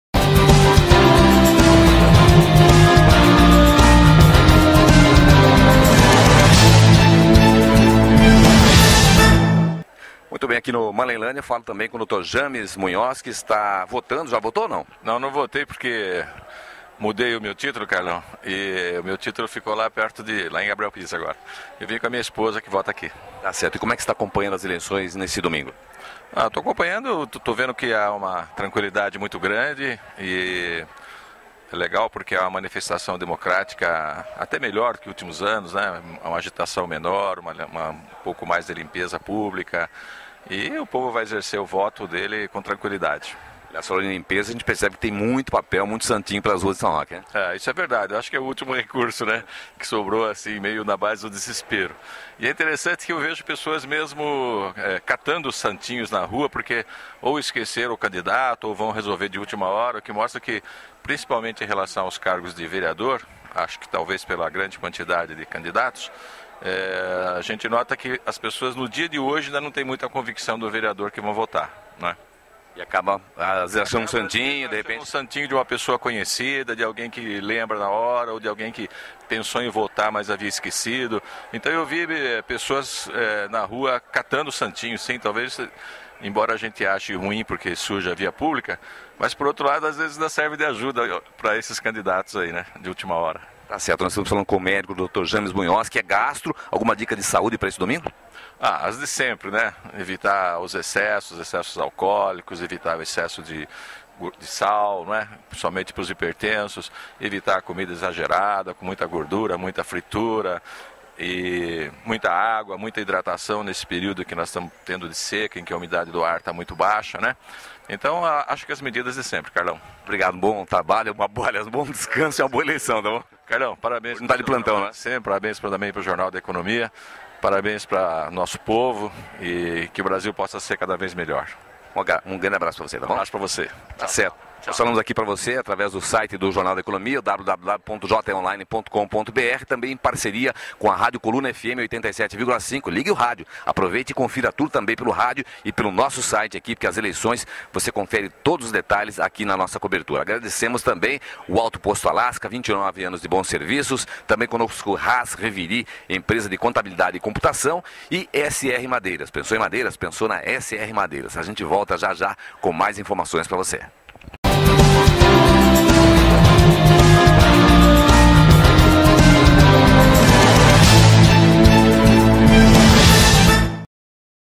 ELEIÇÕES 2012 - A HORA DO VOTO VEJA MAIS REPORTAGEM
Aqui você acompanha mais uma reportagem da TV JE acompanhando a HORA DO VOTO com o repórter Carlos Mello na TV JE.